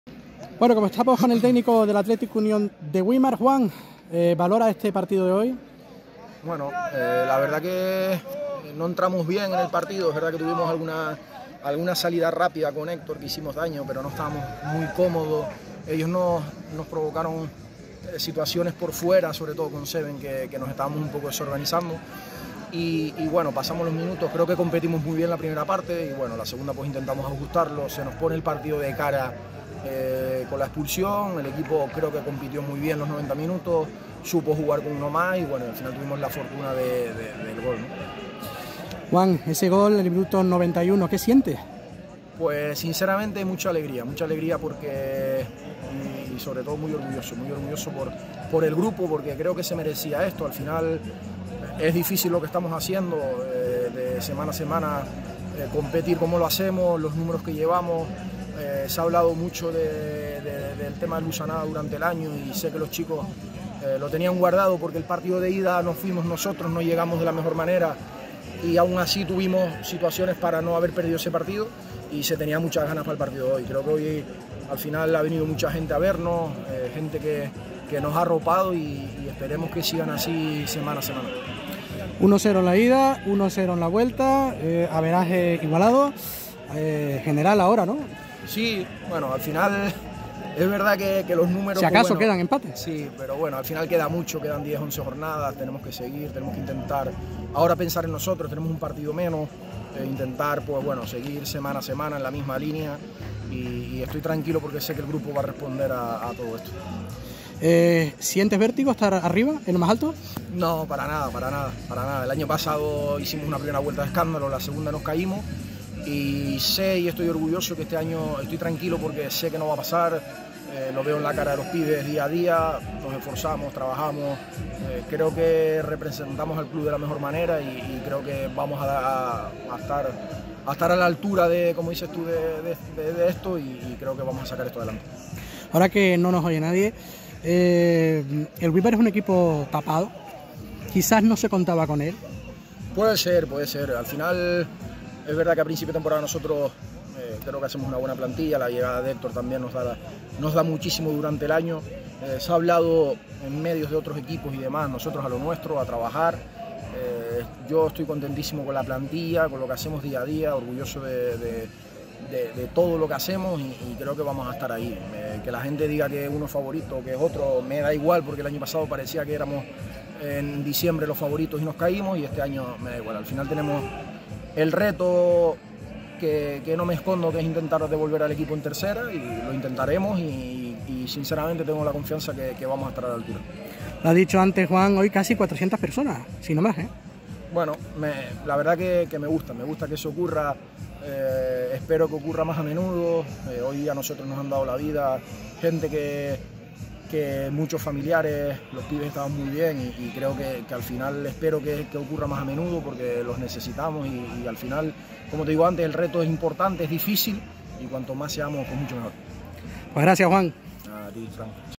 Juan Jesús Alonso tras el partido ante CD Buzanada
Entrevista post partido al entrenador del AUGüímar Juan jesús Alonso tras el aprtido ante el CD Buzanada. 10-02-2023